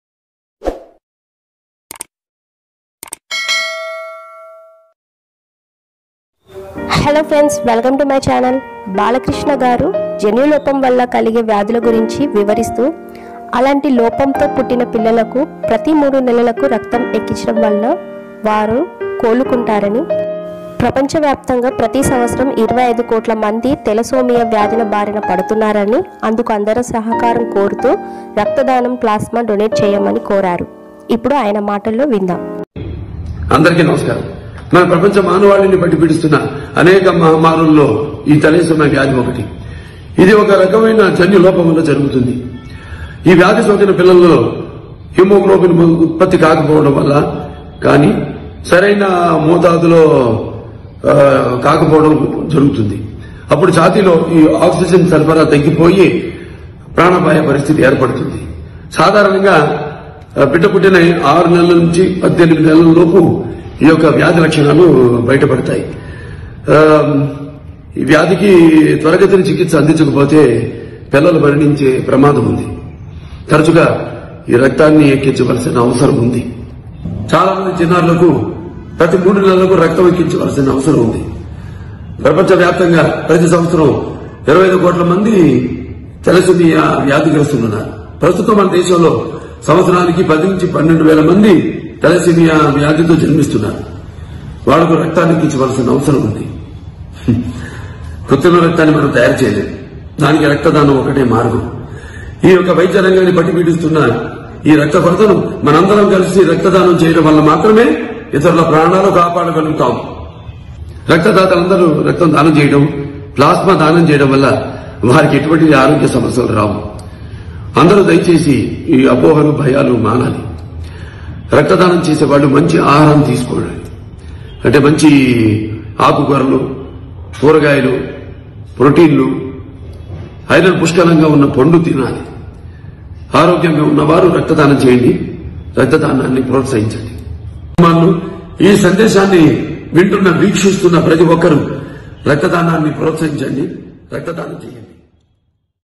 BalaKrishna Garu About Blood Donation Drive||బాలకృష్ణ గారు రక్తదానం చేయాలని "జన్యులోపం"కలిగిన వారిని
NandhamuriBalaKrishna Garu About Blood Donation Drive||బాలకృష్ణ గారు రక్తదానం చేయాలని "జన్యులోపం"కలిగిన వారిని కాపాడాలని కోరారు